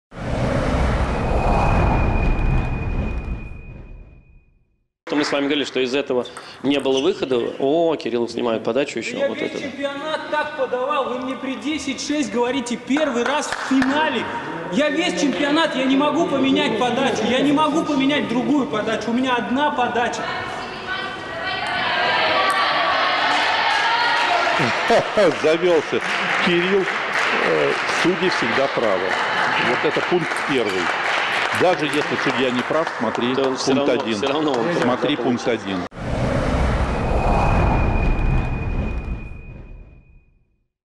У Скачкова снимают «единственную» подачу в финале чемпионата России по настольному теннису
18 апреля 2021 года. Чемпионат России по настольному теннису. Финал.  Мужской одиночный разряд. Кирилл Скачков не скрывает эмоций, когда судья снимает у него подачу в третьей партии финала. Будущий чемпион уверен, что свою «единственную» подачу он исполняет правильно.